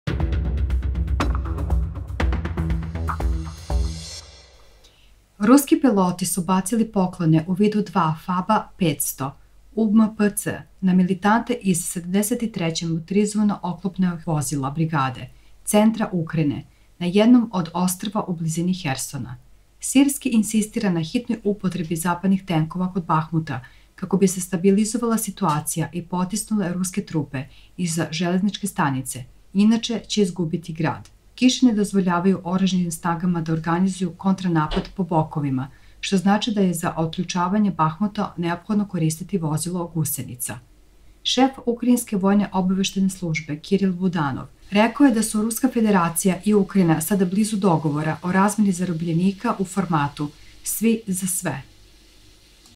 Dva FAB500 UMPC Pogodili Ukrajinske Militante ! Rusija I Ukrajina Rat najnovije Vesti
Ruski piloti su bacili poklone u vidu dva FAB-500 UMPC na militante iz 73. Motorizovano-oklopnih vozila (MTR) centra Ukrajine na jednom od ostrva u blizini Hersona. Sirski insistira na hitnoj upotrebi zapadnih tenkova kod Bahmuta kako bi se stabilizovala situacija i potisnule ruske trupe iza železničke stanice, inače će izgubiti grad. Kiše ne dozvoljavaju Oružanim snagama da organizuju kontranapad po bokovima, što znači da je za otključavanje Bahmuta neophodno koristiti vozila gusenica. Šef ukrajinske vojne obaveštajne službe Kiril Budanov rekao je da su Ruska Federacija i Ukrajina sada blizu dogovora o razmeni zarobljenika u formatu „svi za sve”.

Izvor: Telegram kanal Република Српска